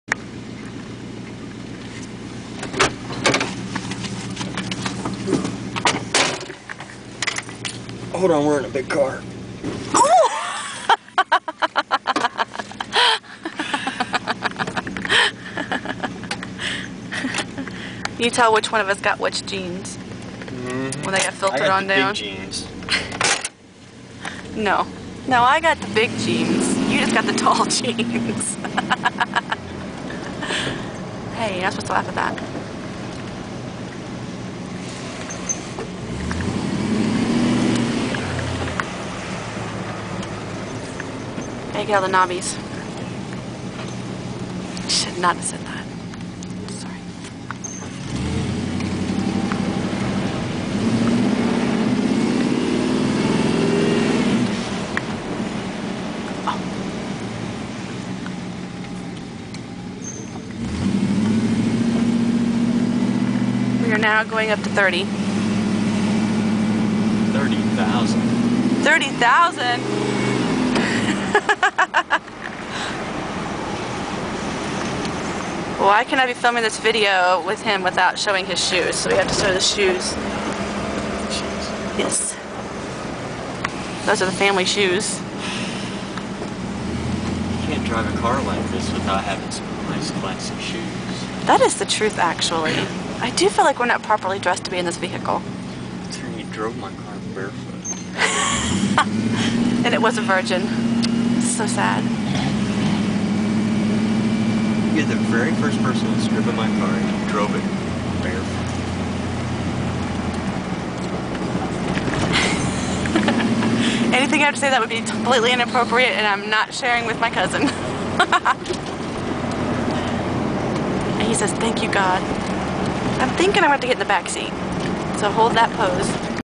1957 Imperial - yeah, 30 thousand!
My super cool cousin Joanna came to visit today. We took the Imperial for a drive.